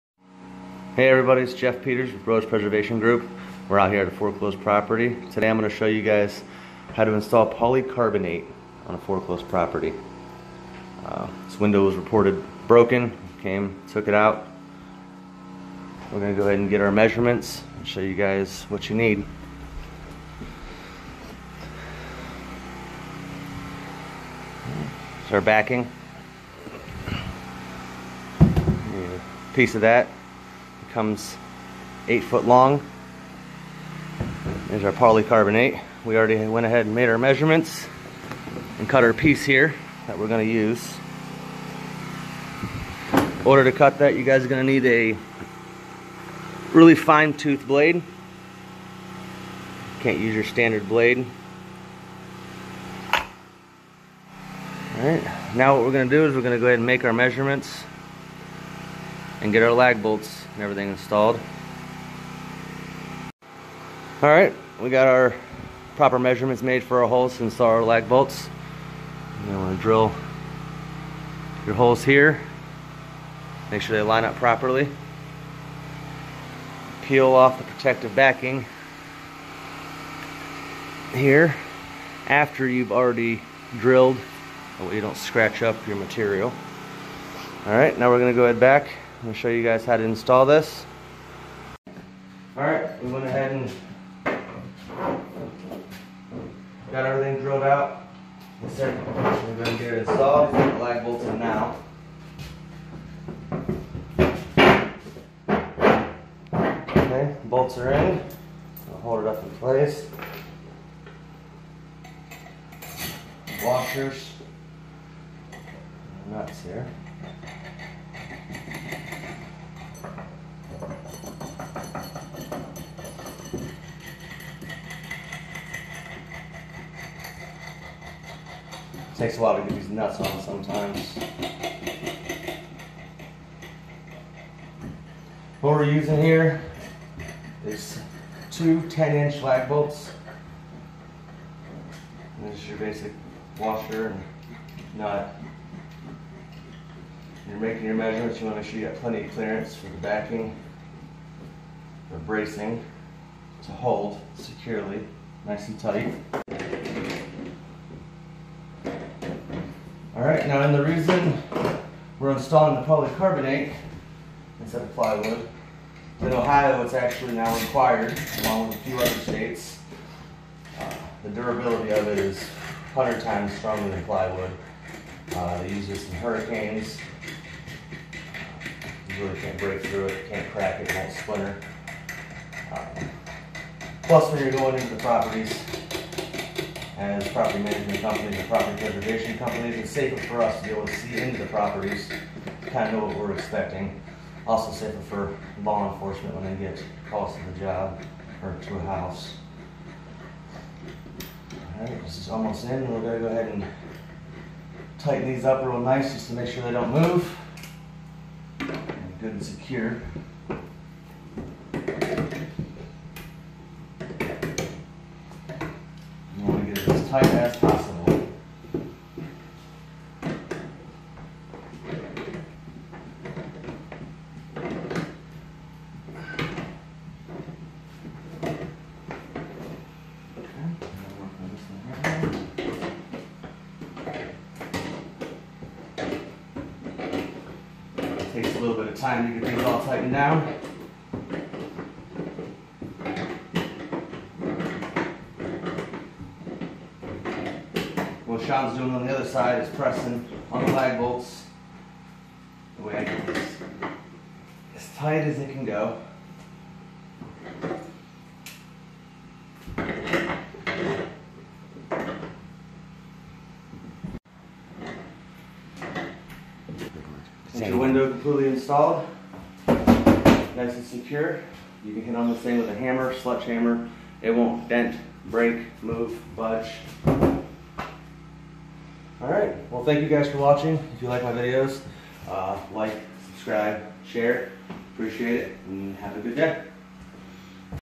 Property Preservation: Securing a window using polycarbonate clearboard!
A quick Tutorial on how to install polycarbonate on a broken window at a foreclosed property!